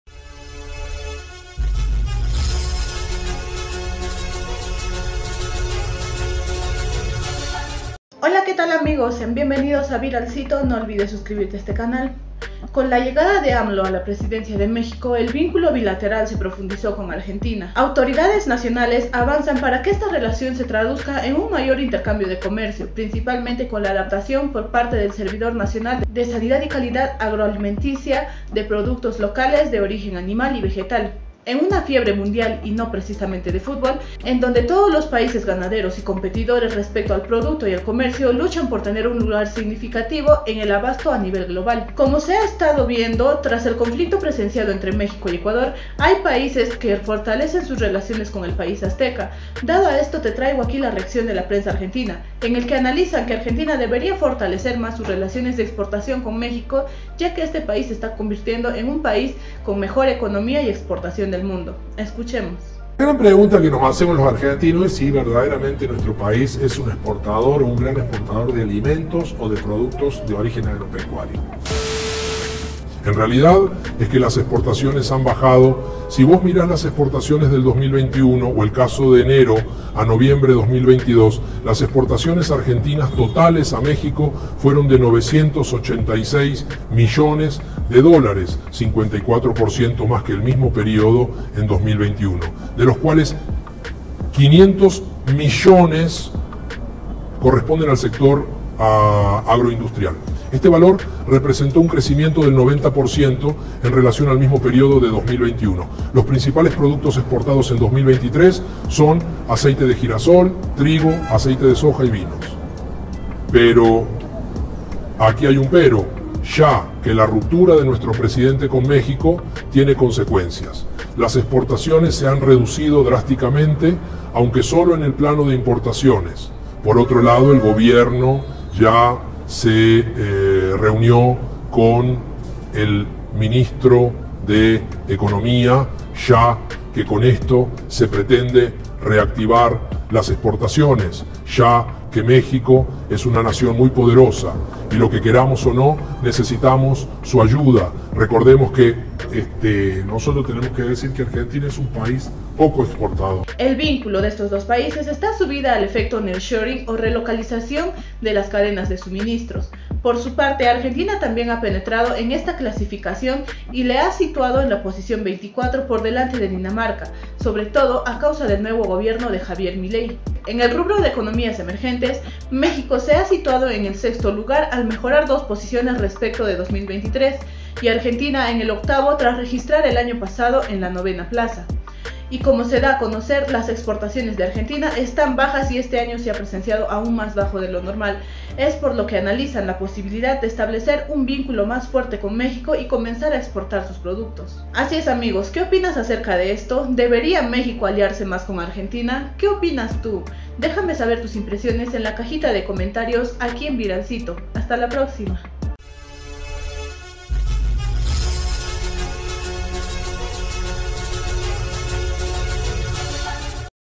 ARGENTINA SE RINDE ANTE MEXICO!! QUEREMOS EXPORTAR A MEXICO Y FORTALECER VINCULOS!!
PRENSA ARGENTINA!! QUEREMOS EXPORTAR A MEXICO Y FORTALECER VINCULOS!! POTENCIA MUNDIAL!